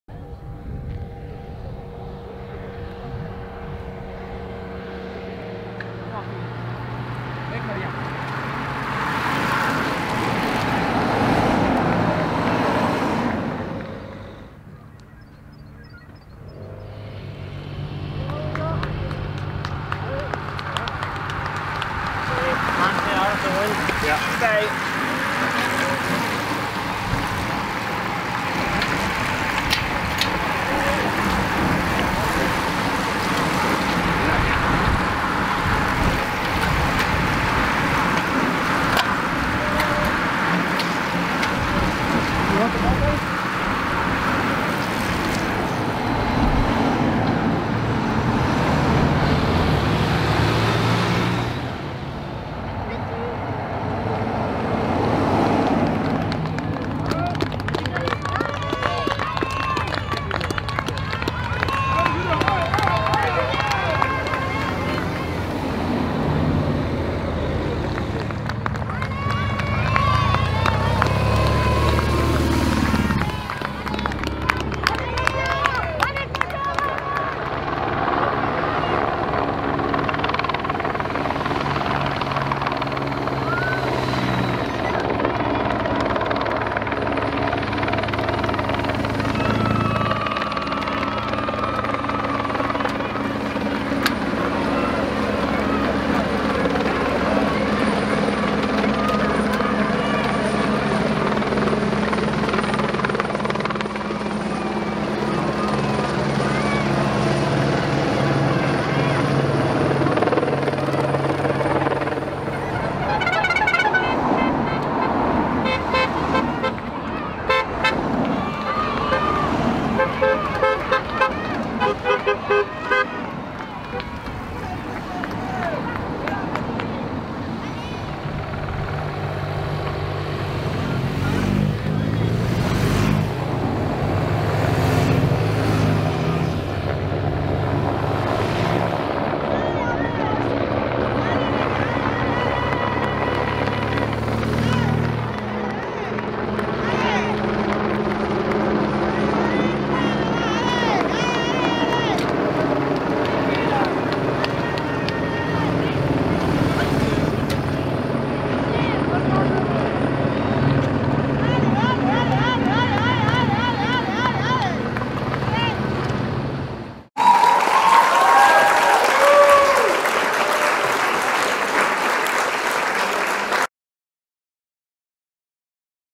PARIS-NICE 4 E ETAPE
4 éme étape entre Chalon sur Saône et Chiroubles. avec les cols et côtes du Mâconnais et beaujolais